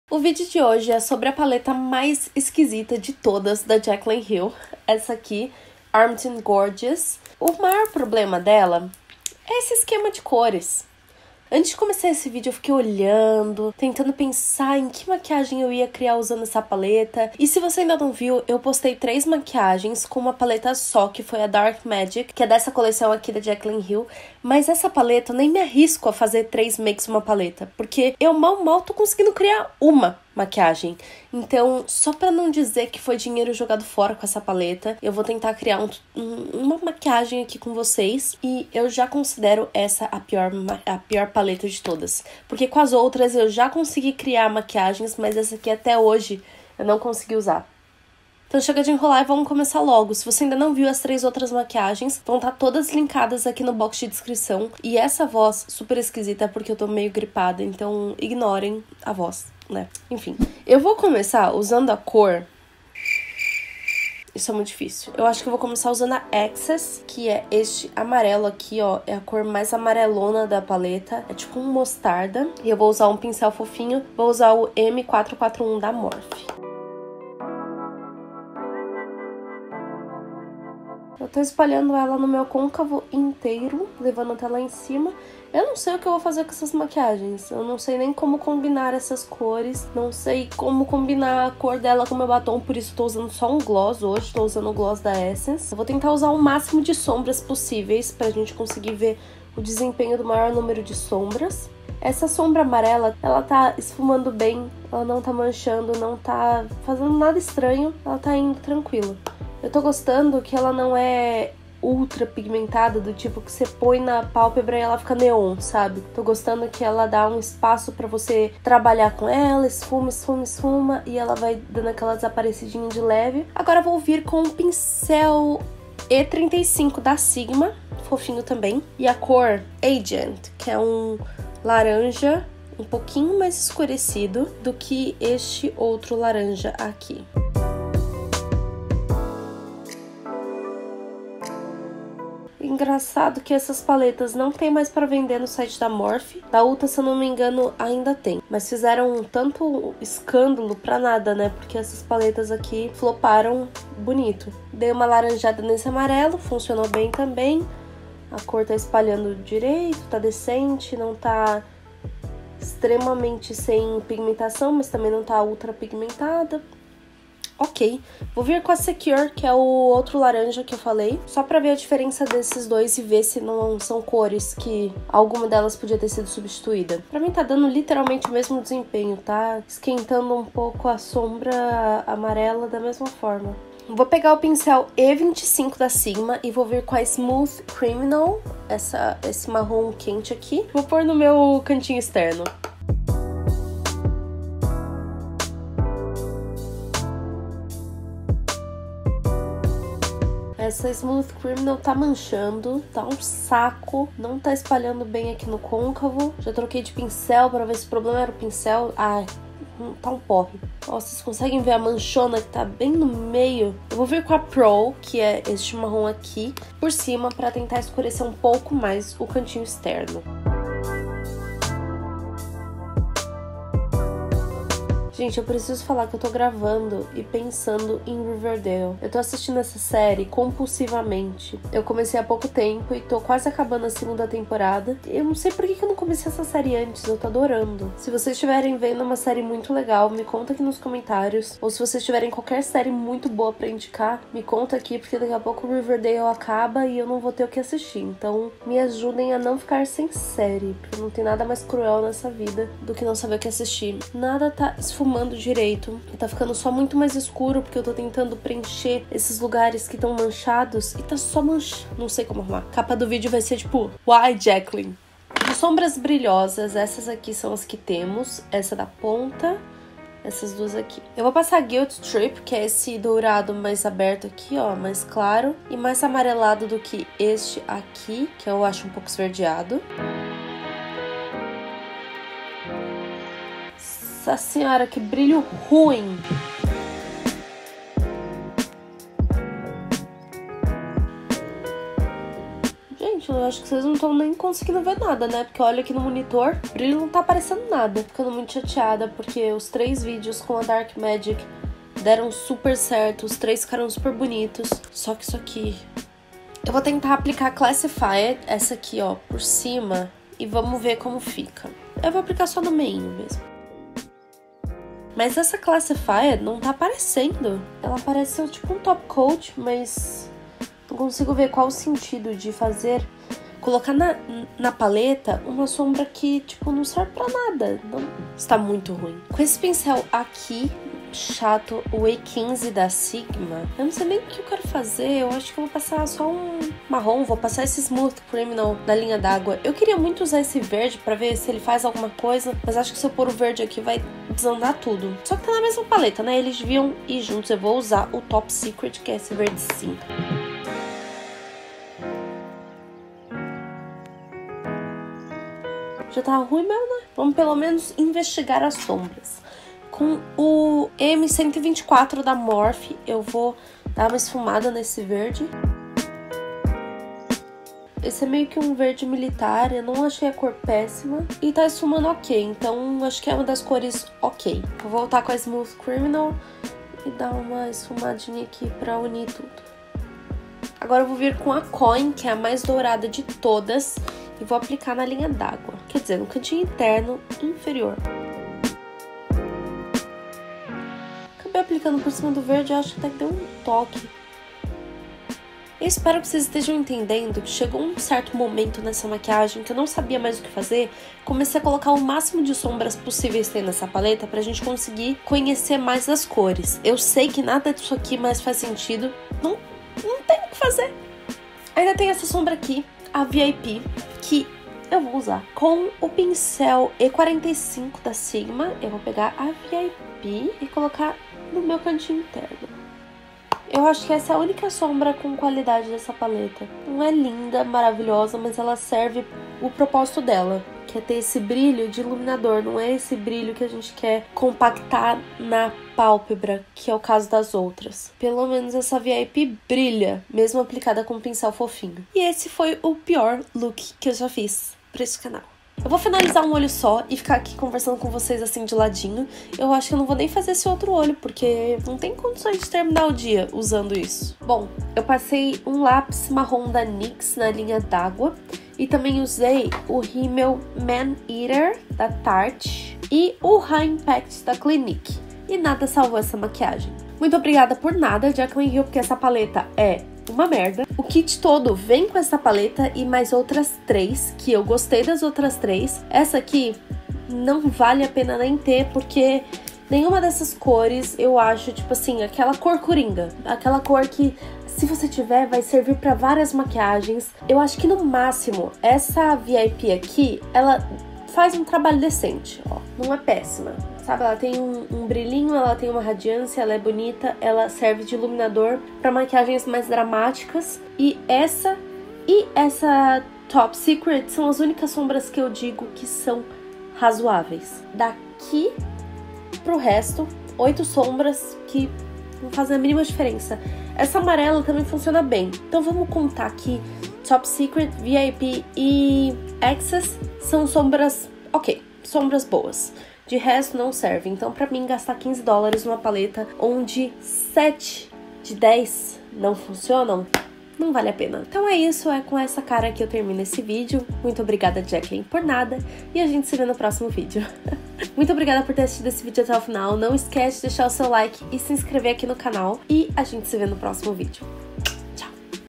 A PIOR paleta de todas! 😱 | Resenha Paleta Armed & Gorgeous
Nesse vídeo eu mostro pra vocês uma das paletas da coleção The Vault da Jaclyn Hill com a Morphe: a paleta Armed & Gorgeous!
Pensa numa paleta ruim... hahahaha

Espero que você goste e não se esqueça de curtir o vídeo!